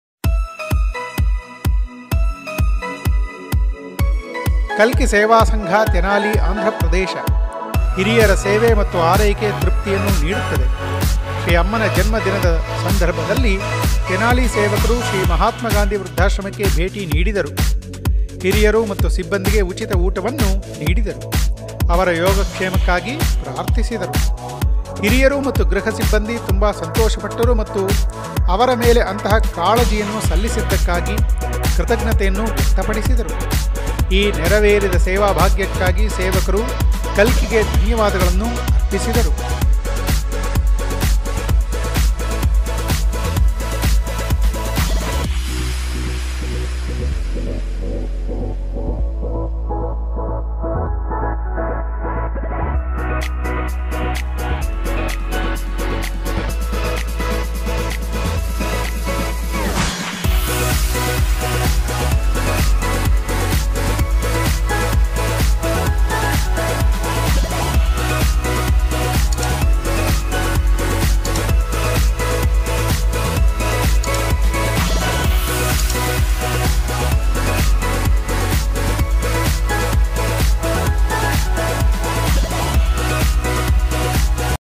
Kannada: Tenali Seva Sangha | On Sri Amma's Janmadina,Visited elders-Provided food & Prayed for them
Kalki Seva Sangha members across length and breath of India helping the needy in many ways. These members are those people who are seekers of enlightenment spreading a wave of Oneness among fellow Indians.